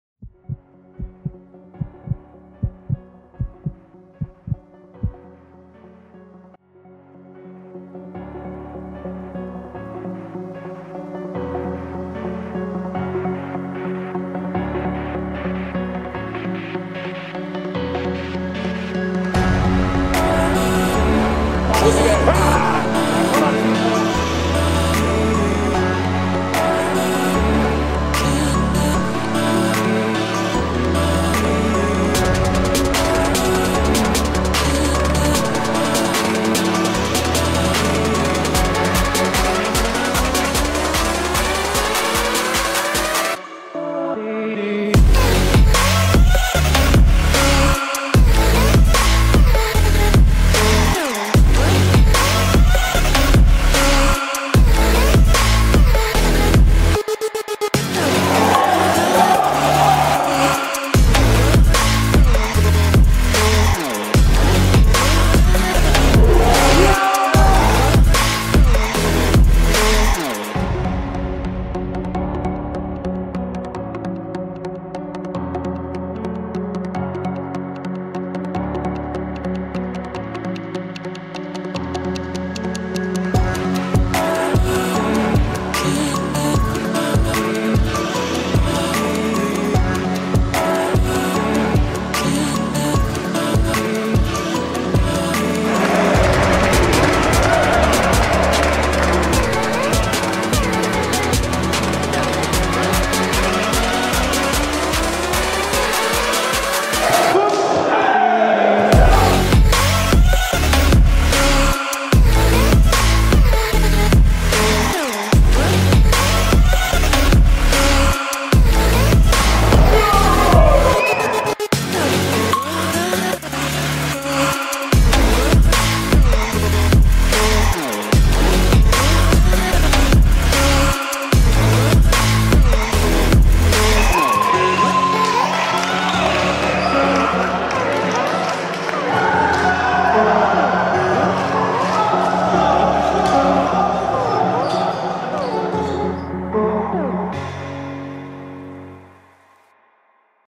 Das große HHU Volleyballturnier [AFTERMOVIE] [YM Release]
In einem packenden Turnier haben 12 Teams von der Heinrich-Heine-Universität um die Volleyball-Krone gekämpft. Doch nur eines konnte sich am Ende krönen! Hier seht ihr das offizielle Aftermovie des großen HHU Volleyballturniers.